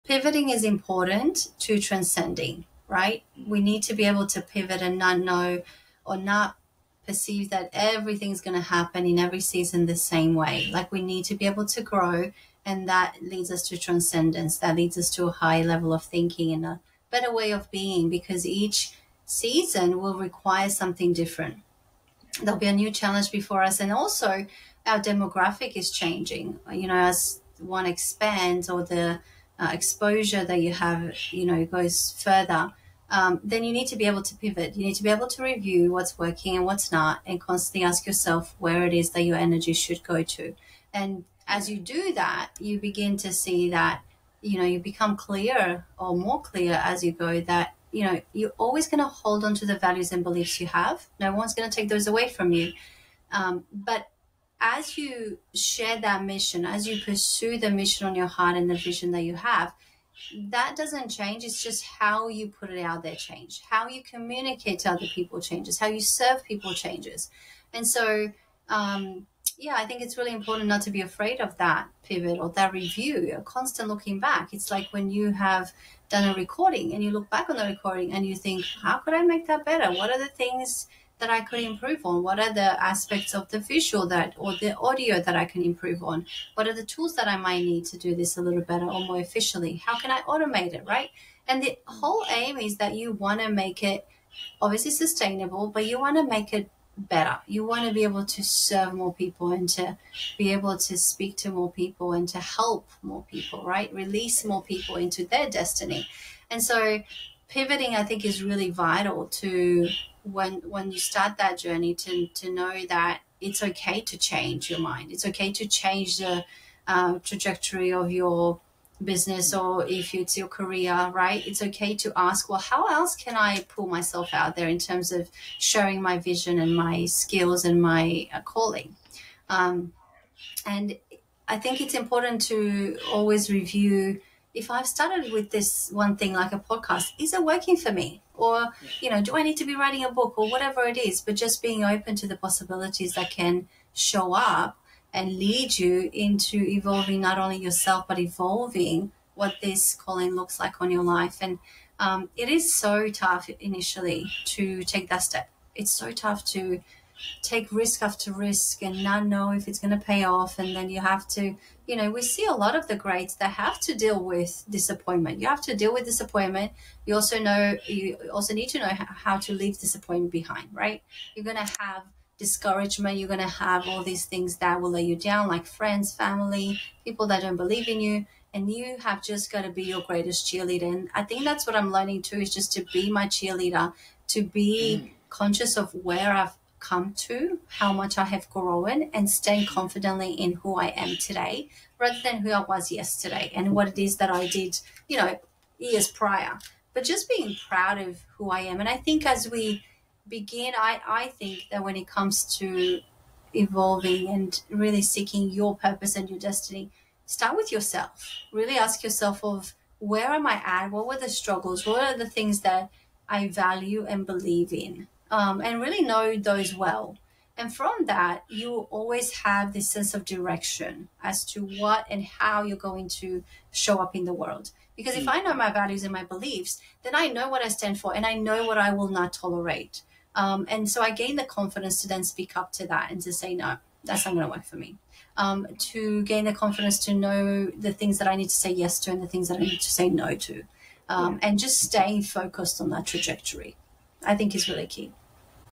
The importance of pivoting and reviewing
@oanabrown2490 talks about the importance of pivoting and reviewing.